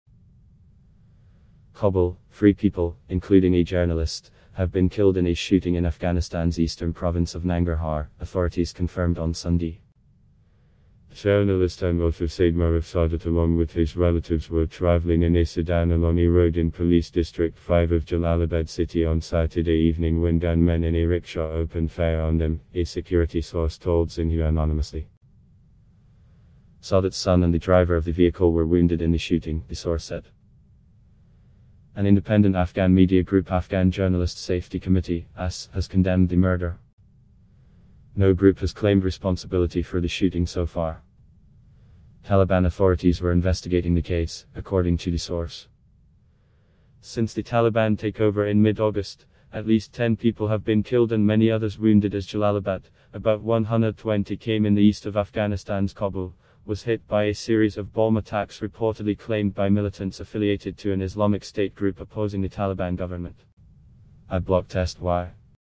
Three including journalist killed in East Afghanistan
KABUL: Three people, including a journalist, have been killed in a shooting in Afghanistan's eastern province of Nangarhar, authorities confirmed on Sunday.